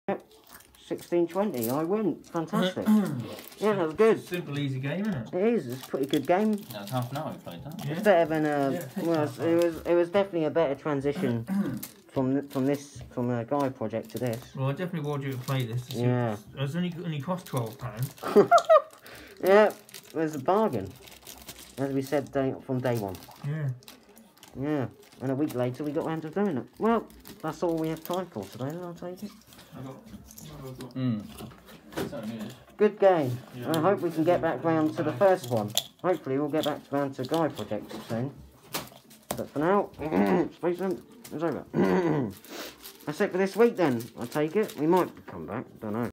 S2 E12 “Gaia Project, Part 1: Phased“ - Part 5! ‘Transition!’ (Spacezone Episode 14)
As we leave Gaia Project, has the struggle of understanding the game ended?!